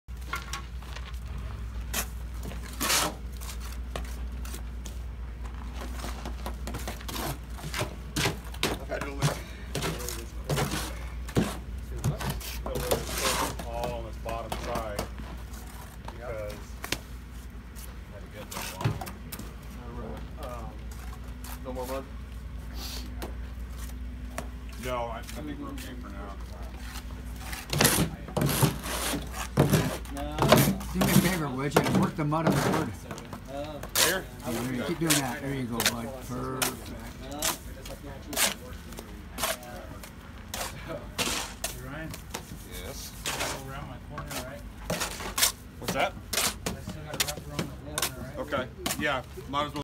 MARSHALLTOWNGATORBACK MORTAR BOARDS GBK10
STOP WASTING TIME AND MORTAR! Give your masons GATORBACK Mortarboards and they’ll both make you money all day! Throw away your plywood boards that suck the water from mortar and decrease your mason’s productivity. GATORBACKs keep mortar workable and consistent to decrease mortar retempering time and waste. Grab a GATORBACK! Pound for pound it’s the lightest, most durable mortarboard that won’t splinter or chip and lasts 3-5 times longer than wood boards.